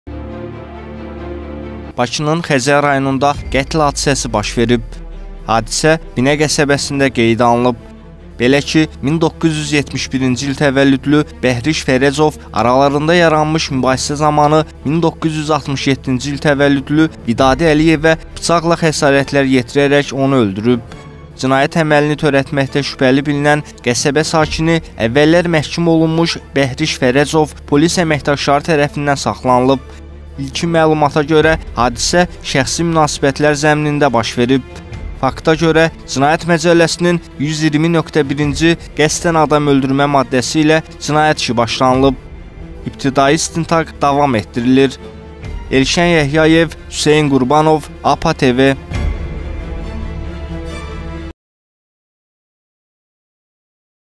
Binədə qətl törədilib - SAXLANILAN VAR – APA TV
Bakının Xəzər rayonunda qətl hadisəsi baş verib.

APA TV xəbər verir ki, hadisə Binə qəsəbəsində qeydə alınıb. Belə ki, 1971-ci il təvəllüdlü Bəhriş Fərəcov aralarında yaranmış mübahisə zamanı 1967-ci il təvəllüdlü Vidadi Əliyevə bıçaqla xəsarətlər yetirərək onu öldürüb.

Cinayət əməlini törətməkdə şübhəli bilinən qəsəbə sakini, əvvəllər məhkum olunmuş Bəhriş Fərəcov polis əməkdaşları tərəfindən saxlanılıb.

İlkin məlumata görə, hadisə şəxsi münasibətlər zəminində baş verib.

Fakta görə Cinayət Məcəlləsinin 120.1-ci (qəsdən adam öldürmə) maddəsi ilə cinayət işi başlanıb.

İbtidai istintaq davam etdirilir.